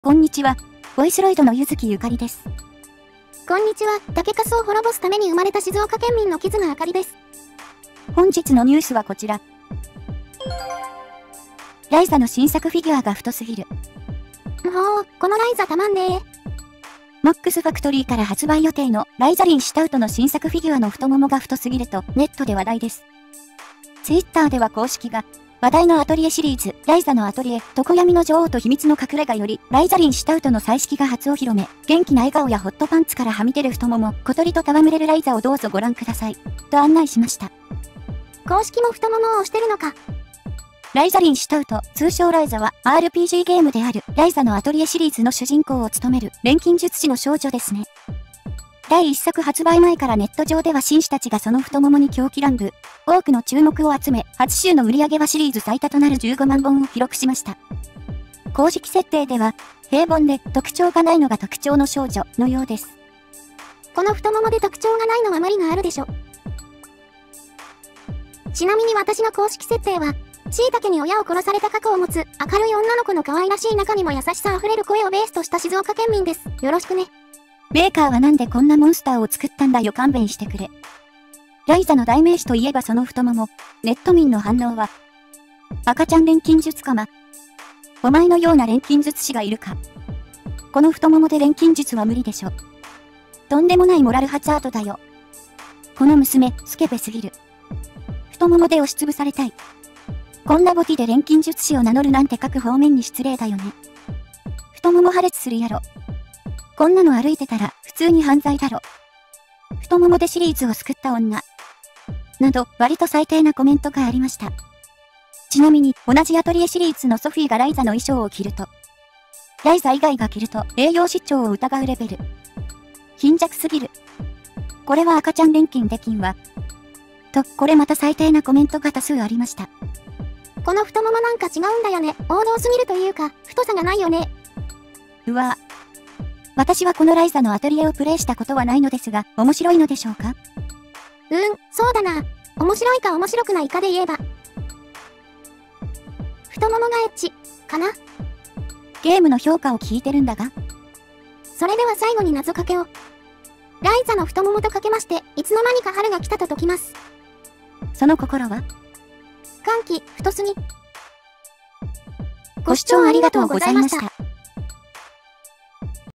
【太もも】ライザの新作フィギュアが太すぎる【VOICEROID】
ライザのキャラデザは言うまでもなく秀逸ですが、２Bのキャラデザには衝撃を受けました。未プレイだけど。
豚カツかニンニクを支援してくれる人
素材はこちらのサイトからお借りしております
みんなもVOICEROID買って動画作ろう！